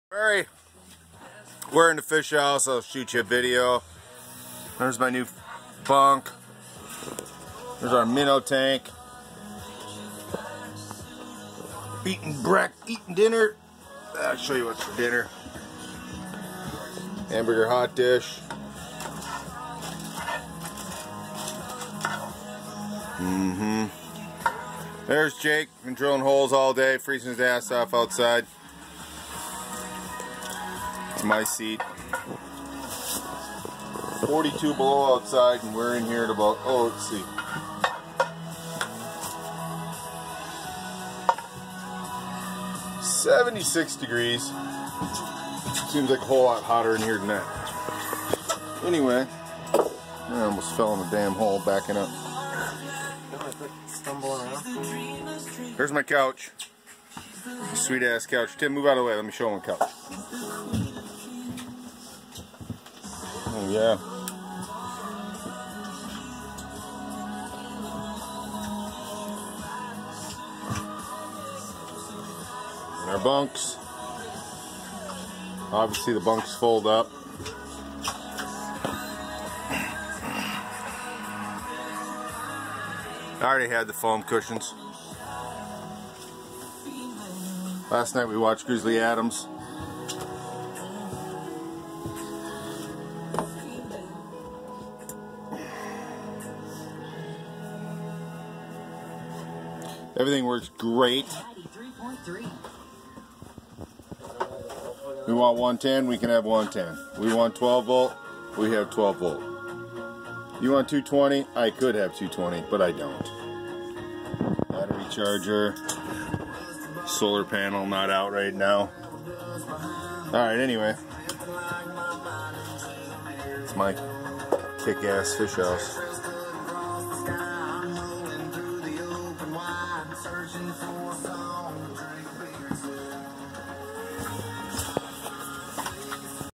First night fishing in our Alaskan addition glacier fish house.
2017 ice fishing Christmas break